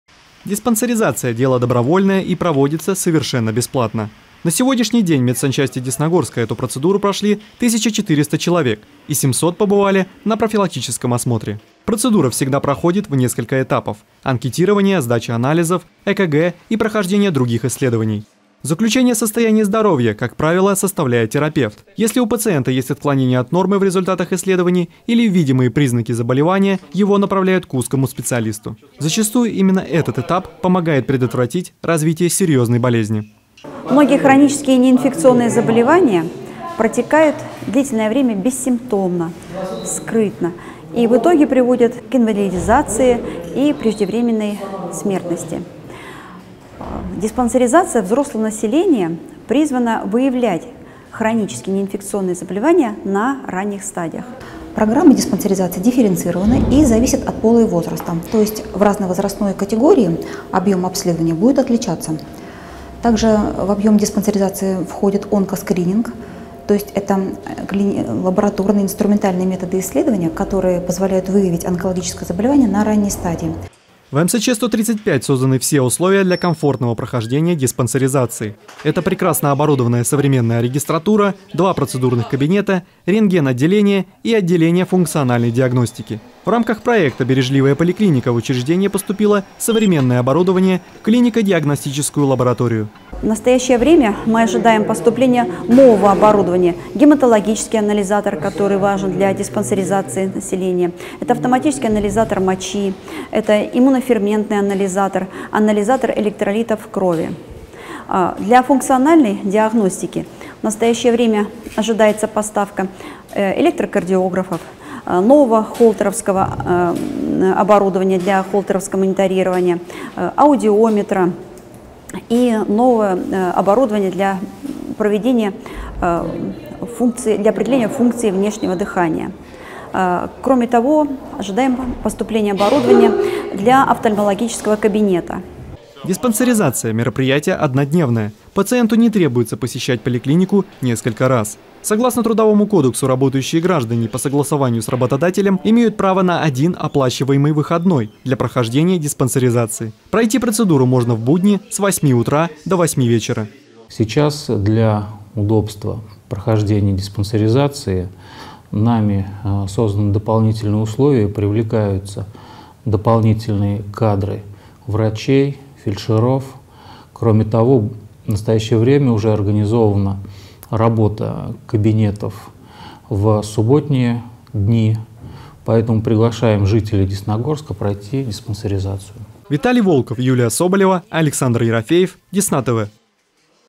Десна-ТВ: «Время проверить здоровье»: МСЧ-135 выводит диспансеризацию на новый уровень
В мае этого года вступил в силу приказ Министерства здравоохранения России об изменении порядка проведения профосмотров и диспансеризации населения. Если раньше она проводилась раз в три года, то по новым правилам такая периодичность будет сохранена только для молодых людей в возрасте от 18 до 39 лет. Начиная с 40 люди должны проходить диспансеризацию каждый год. В медико-санитарной части Десногорска нам рассказали о всех тонкостях прохождения процедуры.